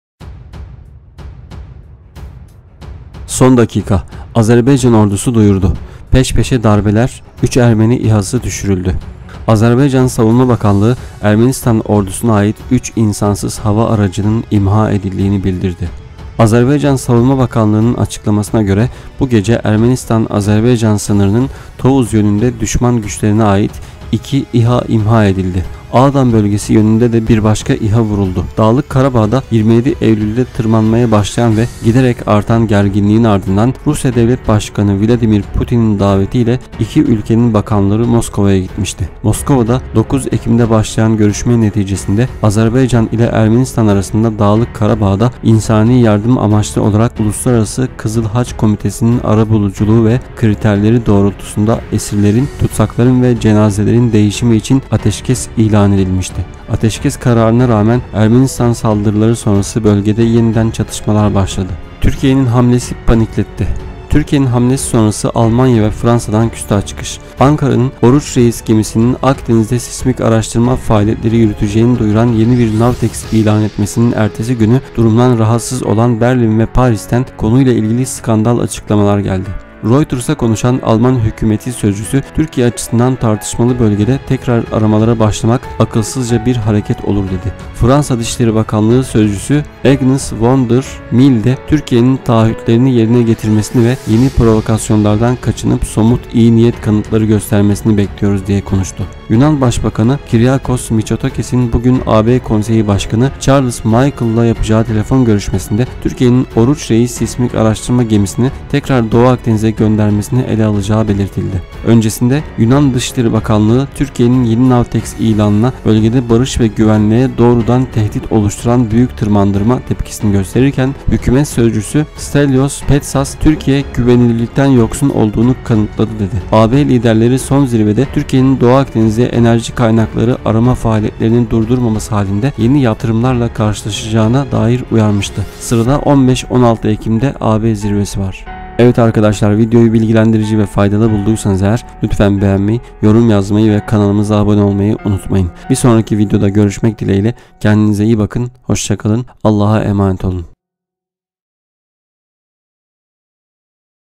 Son dakika: Azerbaycan Ordusu duyurdu! Peş peşe darbeler, 3 Ermeni İHA'sı düşürüldü...Sıcak GELİŞME
Son dakika: Azerbaycan duyurdu! Peş peşe darbeler, 3 Ermeni İHA'sı düşürüldü...
Son dakika haberi: Azerbaycan Savunma Bakanlığı, Ermenistan ordusuna ait 3 insansız hava aracının (İHA) imha edildiğini bildirdi.Savunma Bakanlığı'nın açıklamasına göre, bu gece Ermenistan-Azerbaycan sınırının Tovuz yönünde düşman güçlerine ait iki İHA imha edildi. Ağdam bölgesi yönünde de bir başka İHA vuruldu.

Dağlık Karabağ’da 27 Eylül’de tırmanmaya başlayan ve giderek artan gerginliğin ardından, Rusya Devlet Başkanı Vladimir Putin’in davetiyle iki ülkenin bakanları Moskova’ya gitmişti.

Moskova’da 9 Ekim’de başlayan görüşme neticesinde Azerbaycan ile Ermenistan arasında Dağlık Karabağ’da, insani yardım amaçlı olarak Uluslararası Kızılhaç Komitesi'nin arabuluculuğu ve kriterleri doğrultusunda esirlerin, tutsakların ve cenazelerin değişimi için ateşkes ilan edilmişti.

Ateşkes kararına rağmen Ermenistan saldırıları sonrası bölgede yeniden çatışmalar başladı.

Yedi Gündem..